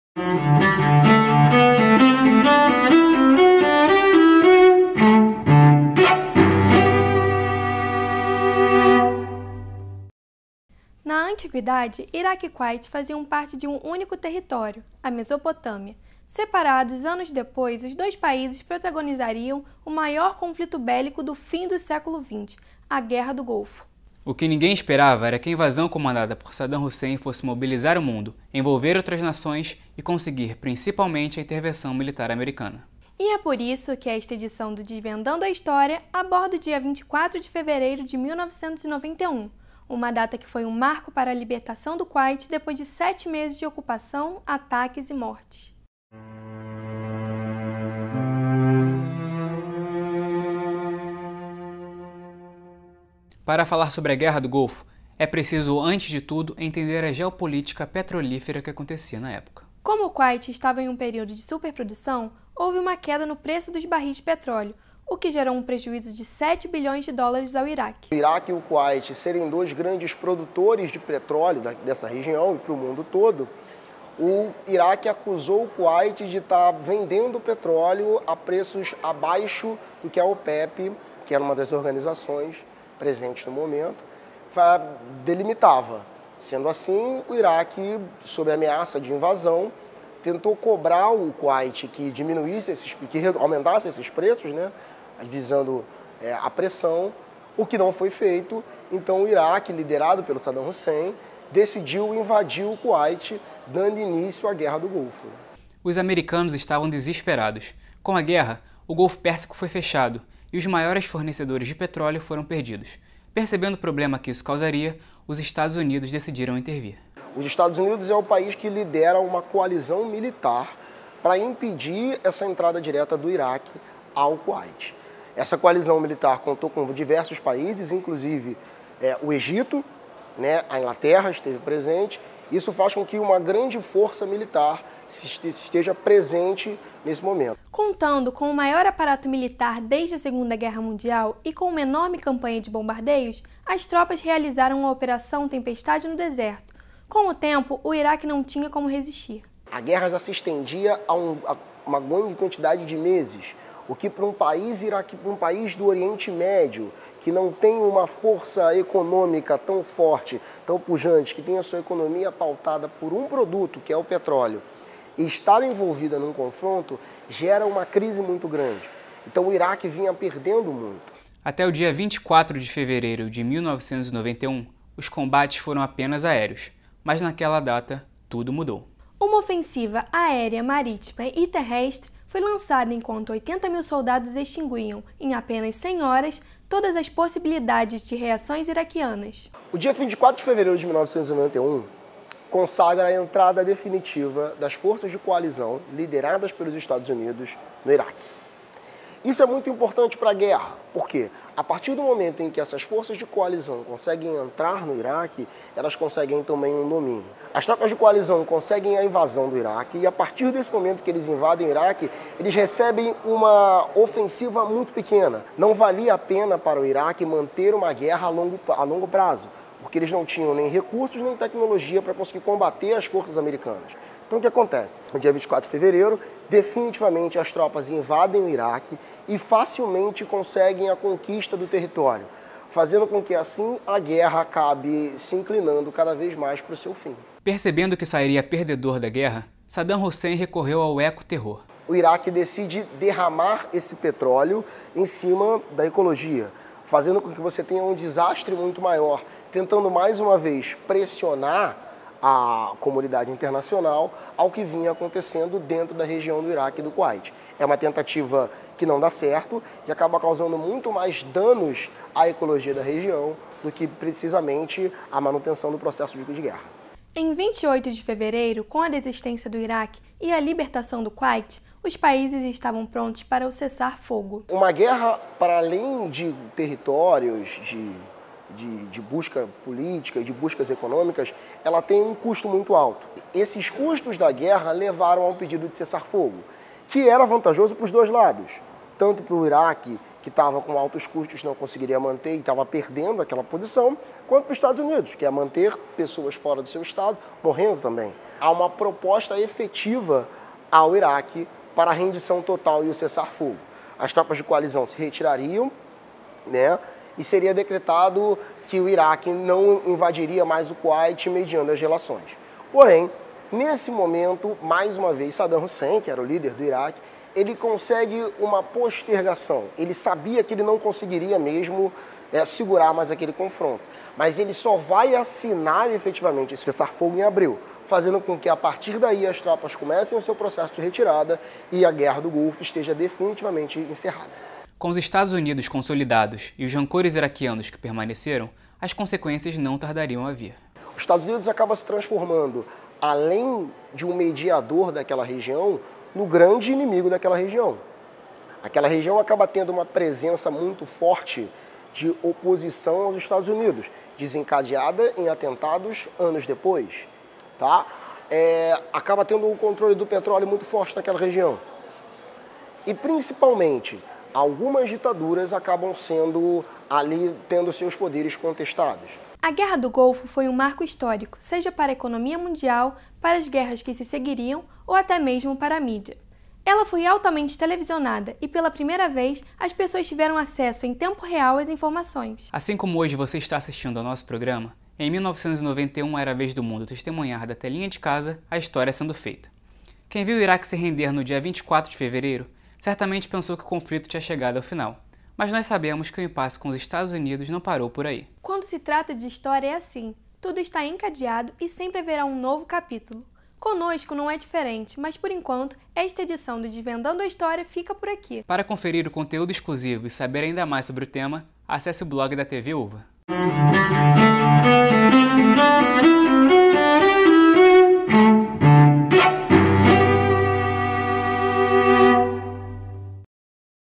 TV UVA - Desvendando a História - Guerra do Golfo
Em 24 de fevereiro de 1991, os Estados Unidos iniciaram uma ofensiva terrestre contra o Iraque devido à invasão do Kuwait. Esse marco histórico foi fundamental para o fim da Guerra do Golfo e esta edição do "Desvendando a História" conta o que aconteceu.